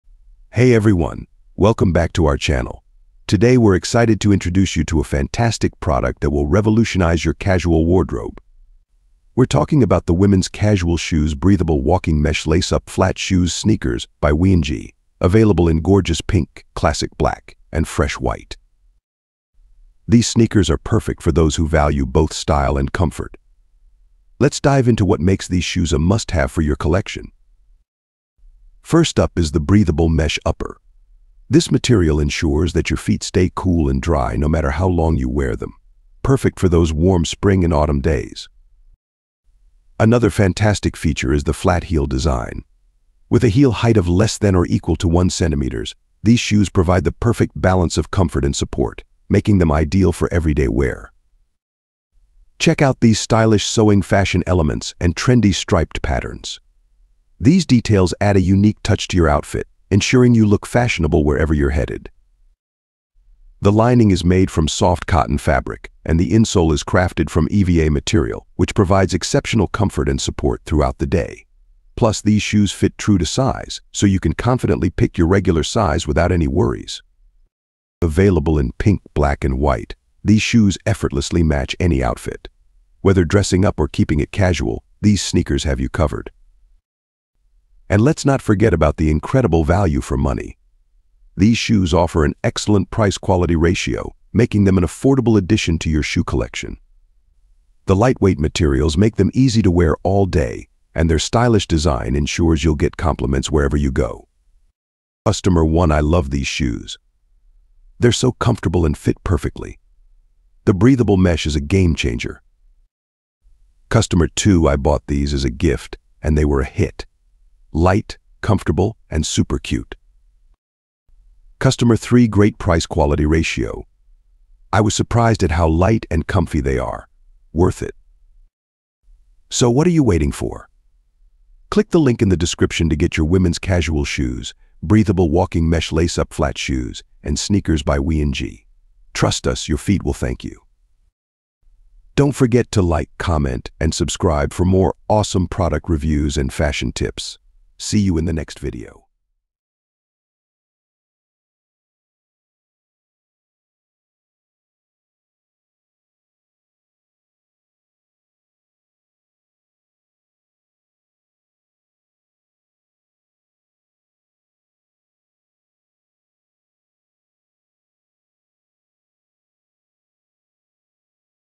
WIENJEE Women Casual Shoes: Ideal for Everyday Wear
⭐ = Make WIENJEE Women Casual Shoes your ideal choice for everyday wear. With their breathable mesh material and comfortable flat heel, these sneakers are perfect for long walks, errands, and casual outings. Enjoy the perfect blend of comfort and style with these versatile shoes.

𝑽𝒊𝒅𝒆 𝑬𝒅𝒊𝒕𝒐𝒓: Abdullah As Sayem.
𝑽𝒐𝒊𝒄𝒆: Yubair Hossain.
𝑺𝒄𝒓𝒊𝒑𝒕: Atiqur Rahman.